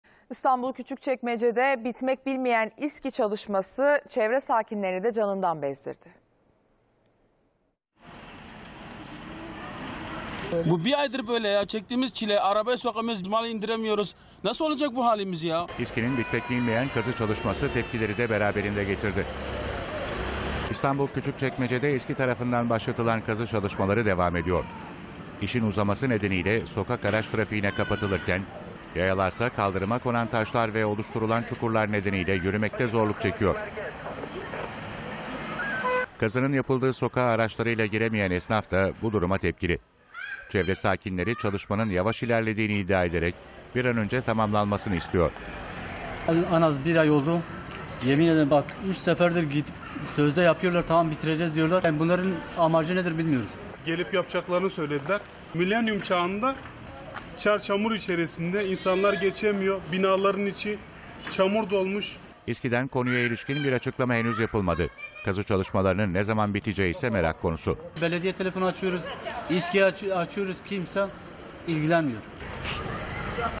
Küçükçekmece’de bitmeyen İSKİ çilesi! | A Haber
İstanbul Küçükçekmece’de 1 aydır bitmeyen İSKİ çalışması vatandaş ve çevre esnafın tepkisine yol açtı. Esnaf, çalışmalar nedeniyle araçlarıyla sokağa giremezken, kadınlar ise çocuk arabalarıyla yolda yürürken adeta çile çekiyor.

Küçükçekmece Kanarya Mahallesi Huzur Sokak üzerinde yaklaşık 1 aydır, İSKİ tarafından başlatılan kazı çalışmaları devam ediyor. Bitirilemeyen çalışmalar nedeniyle sokak araç trafiğine kapatılırken, vatandaşlar ise kaldırıma konan taşlar ve oluşturulan çukurlar nedeniyle yürümekte zorluk çekiyor. Kazının yapıldığı sokakta araçlarıyla giremeyen esnaf duruma tepki gösterdi. Bebek arabalarıyla sokakta yürümeye çalışan kadınlar ise adeta çile çekiyor. Çevre sakinleri ise çalışmaların yavaş ilerlediğini iddia ederek bir an önce sona ermesini istedi.

Sokakta tekstil işiyle uğraşan ve ürün getirip götüren esnaf Mehmet Bora, “Bu bir aydır böyle, çektiğimiz çile. Arabayı sokamıyoruz, mal indiremiyoruz nasıl olacak bu halimiz. Sürekli böyle yıkıyorlar, yapıyorlar tekrar bozuyorlar, tekrar yapıyorlar. İSKİ yapıyor, kim yapıyorsa bizi rezil ettiler. Paraları çok herhalde böyle yapıyor. Millet rezil oldu, ne zaman gelsem böyle, arabamız var ama malımızı getiremiyoruz buraya” diye konuştu.
Yemek işini yaptığını tozdan ve çamurdan kapıyı açamadığını anlatan esnaf Selman Ece, “Bu en az bir ay oldu, sözde yapıyorlar. Gidiyorlar yukarıya kadar, gelip baştan bir daha yıkıyorlar, bunların amacı nedir bilmiyoruz. Soruyoruz, yetkili kim tam olarak ne zaman bitecek bilelim. Yetkili yok diyorlar. Bir hafta geçiyor bir daha baştan başlıyorlar. Perişan olmuşuz, iş yerimizi yemek götürüyoruz, tekstiller var. burada kavga bile oldu bu yüzden, iki üç tane kaza oldu. Bu sabah geldim arabam kaldı, iki üç kişi geldi zor arabayı çıkardık. Millet telefon açıyor, İSKİ’ye açıyor, kimse ilgilenmiyor. Kim ilgileniyorsa lütfen bir an önce bu mağduriyeti gidersin” ifadelerini kullandı.

Evinde yaşlı ve hasta insanların olduğunu doktora götürürken sokağın başına kadar yürütmek zorunda kaldığını sokakta ikamet eden Servet Aslan isimli vatandaş ise, “En az iki üç sefer bunu yetkililere söyledik, gelip yapacaklarını söylediler. Milenyum çağında çar çamur içerisinde, insanlar geçemiyor, binaların içi çamur dolmuş. Her halde bir on sefer şikayet ettim, her defasında yetkililere söyleneceği söylendi ama hiçbir sonuç alamadık. Ortalama 1 aydır böyle, geçemiyoruz buradan, normal şartlarda yürüyemiyoruz. Yaşlılar var, bebek arabası olan insanlar var, kadınlar çocuklar var. Ne arabalar geçebiliyor, ambulans dahi giremez şu sokağa” dedi.